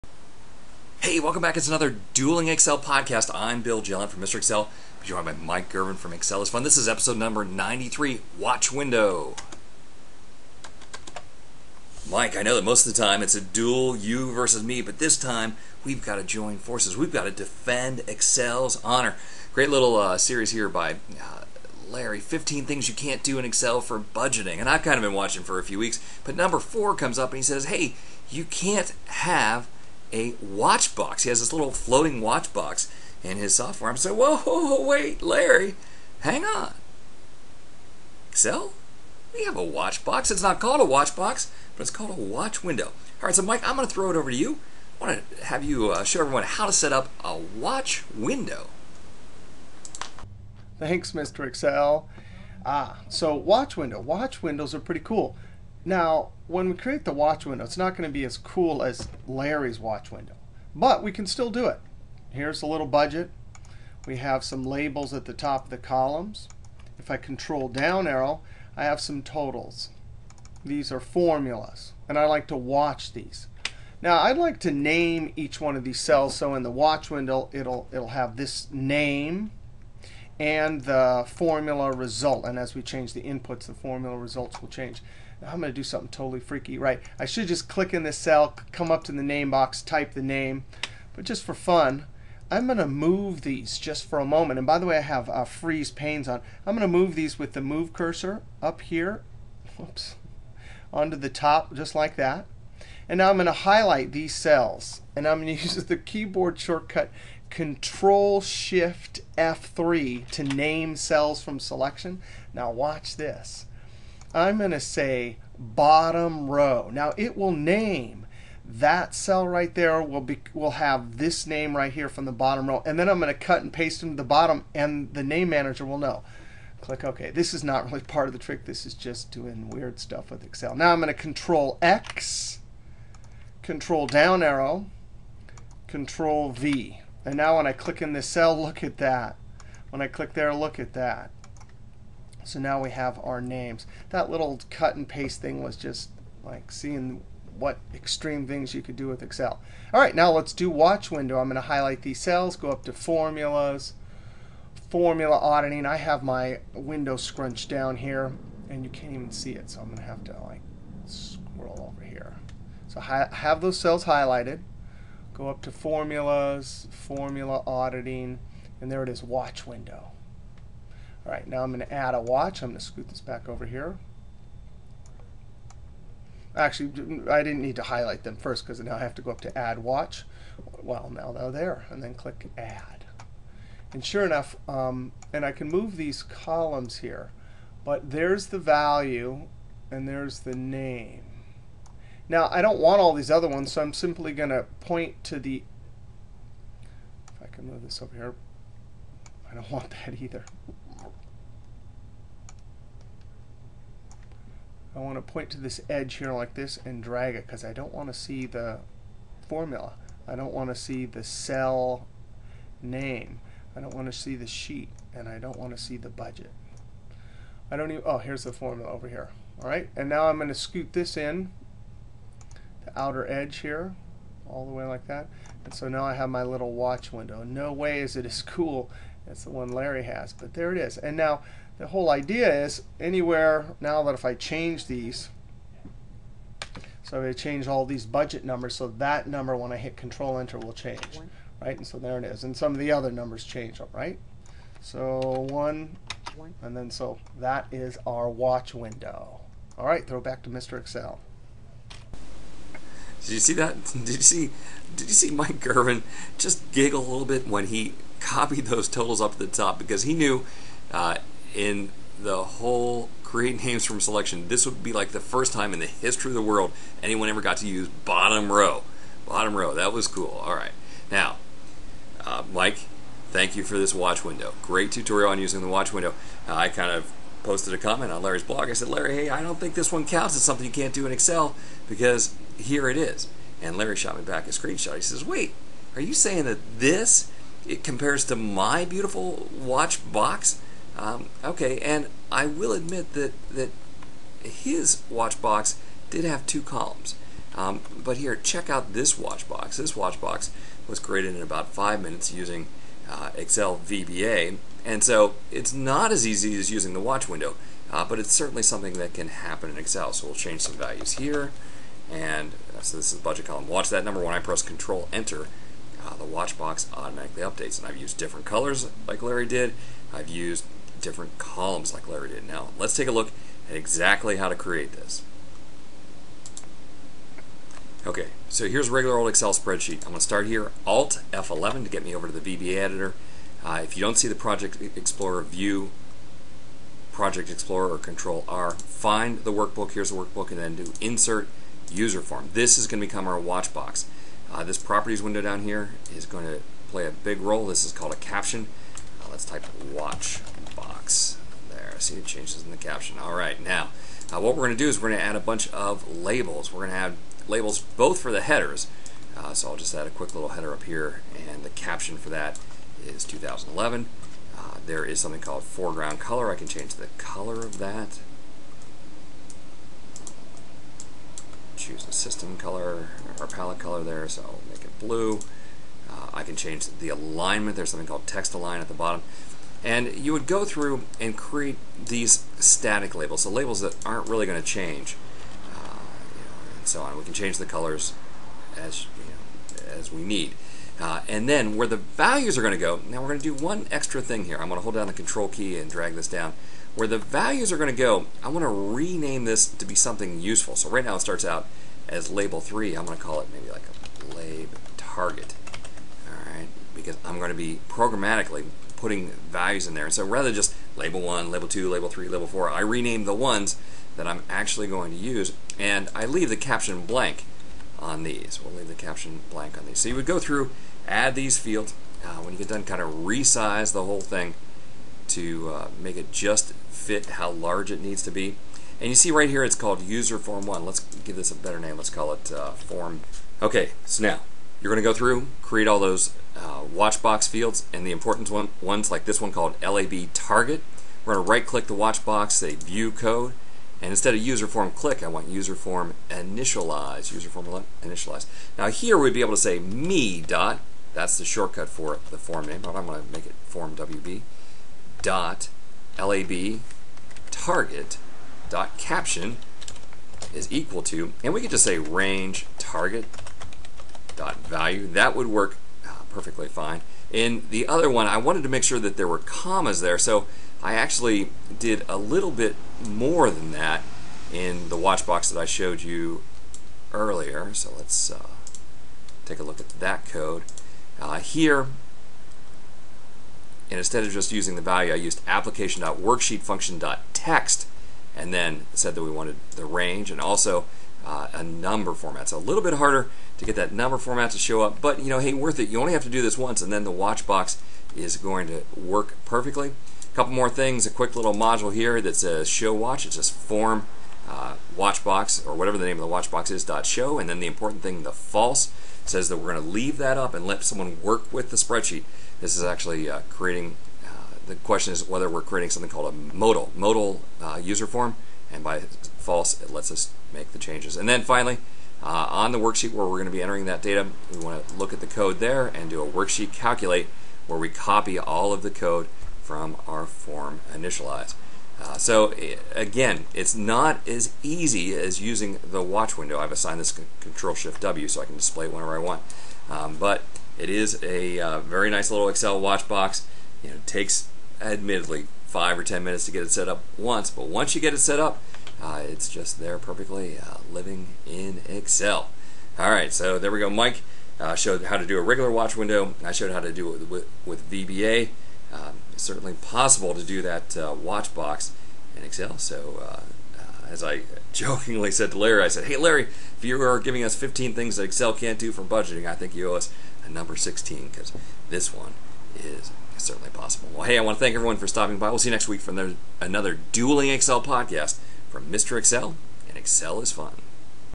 Mr Excel & excelisfun Trick 93: Watch Window: VBA or Watch Window
See Mr Excel and excelisfun use two methods to create a Wacth Window